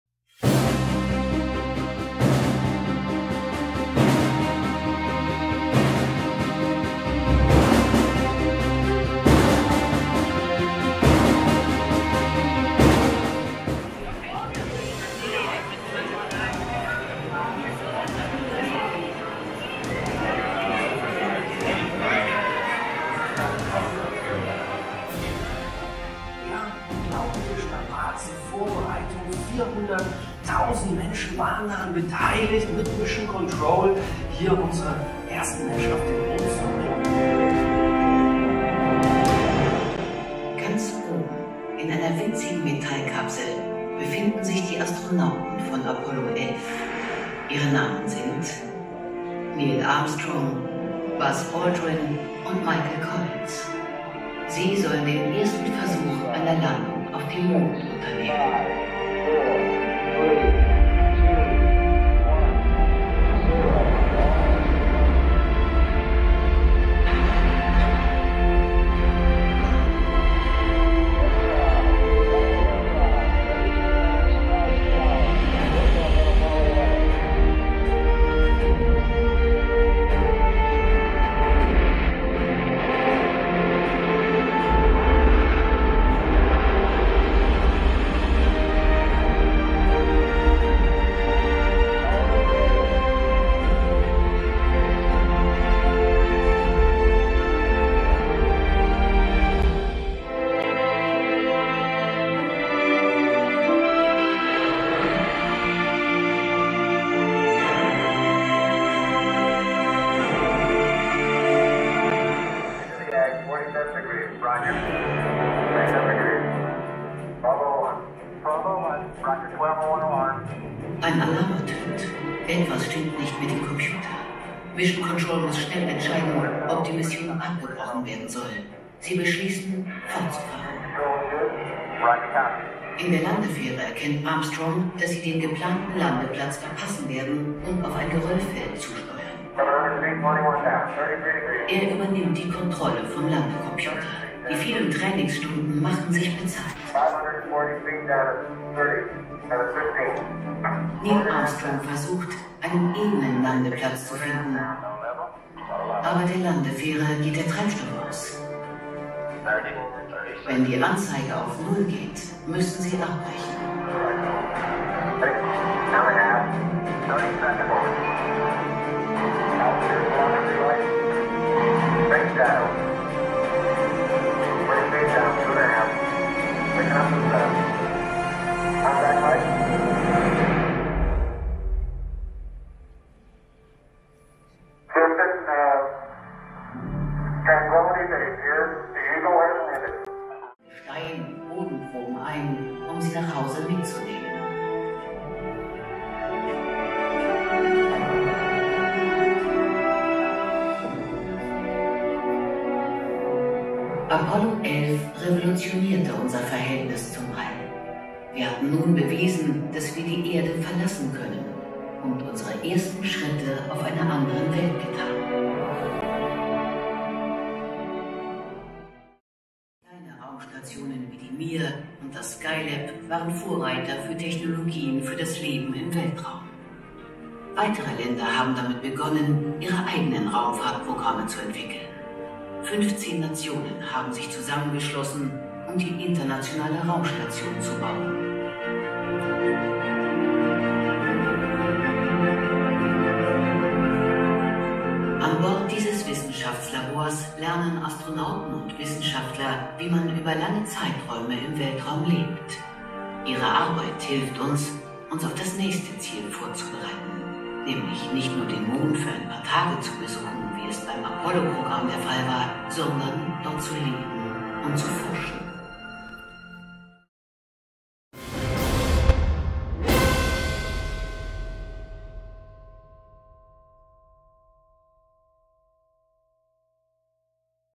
Vor 50 Jahren: "Der Adler ist gelandet." Apollo 11 auf dem Mond.
Es scheint, als wäre man dabei gewesen... Rückschau und Ausblick mit einer fantastischen 8k-Animation im Zeiss-Großplanetarium in Berlin.